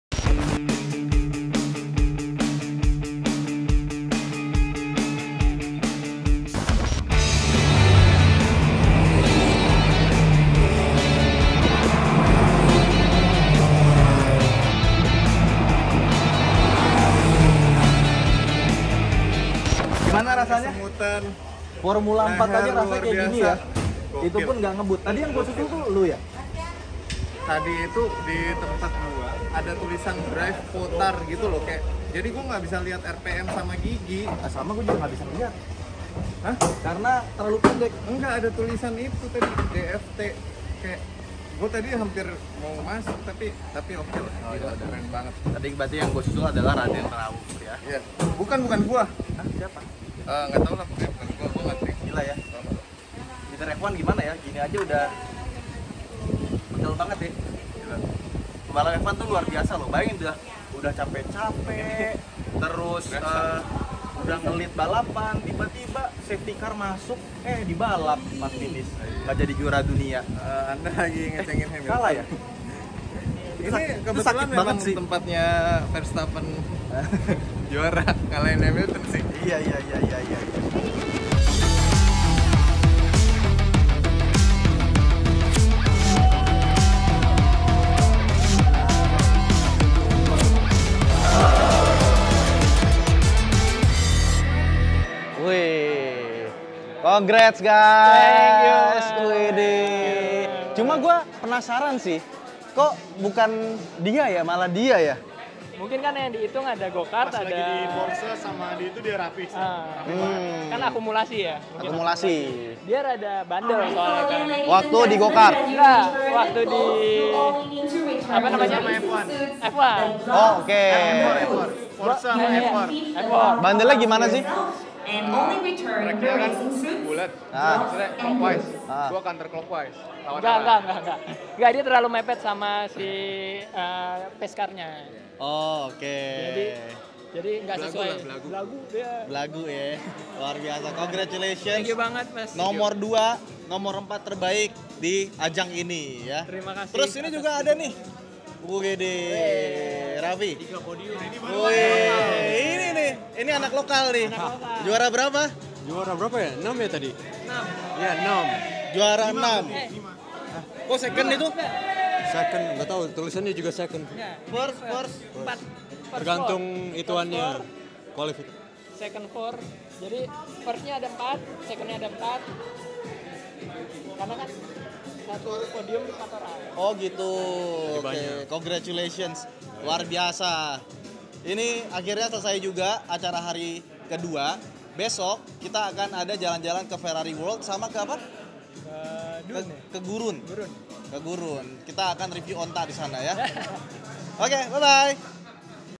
PART 2 : MENCOBA MOBIL FORMULA 4 DI SIRKUIT YAS MARINA!
Apakah Ridwan Hanif berhasil naik podium di Motul Driving Experience Abu Dhabi? Tonton videonya sampai habis ya!